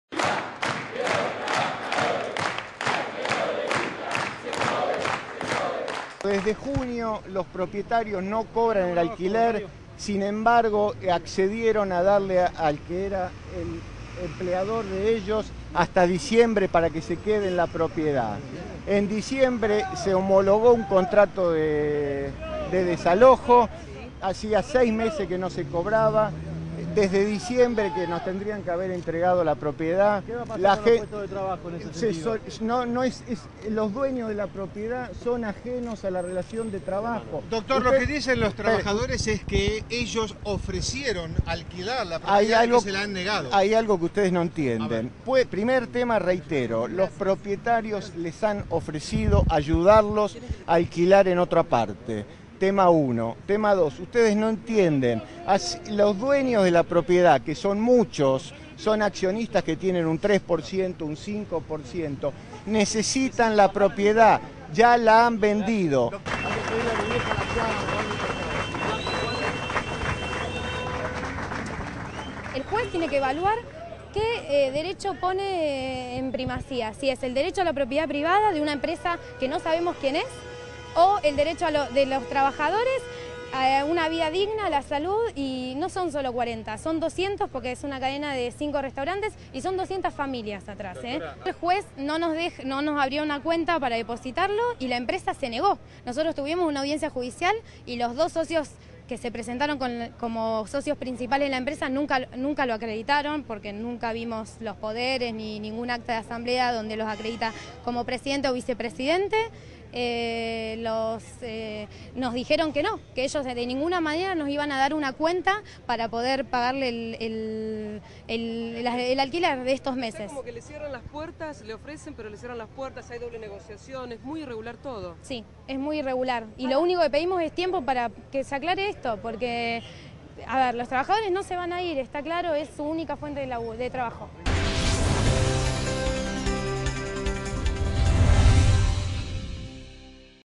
Trabajadores de Cooperativa Alé Alé, rechazan orden de desalojo
Los trabajadores constituidos en cooperativa, que gestionan la parrilla Alé Alé, del barrio porteño de Villa Crespo, rechazaron esta mañana la orden de desalojo emitida por el Juez en lo civil, Martín Christello, presentada por el oficial de Justicia y el abogado de la empresa propietaria del local, donde funciona el restaurante.

Pablo Holman, abogado de los propietarios, aseguró que "desde diciembre que tendrían que haber entregado la propiedad y los dueños la necesitan porque está vendida"

En tanto, Ornella Nociti, letrada de la Federación que nuclea a los cooperativistas, explicó que "el juez deberá decidir que prima, si la propiedad privada o el derecho de los trabajadores" y agregó "nosotros lo que le pedimos al juez es tiempo, para poder mudarnos a otro local" concluyó.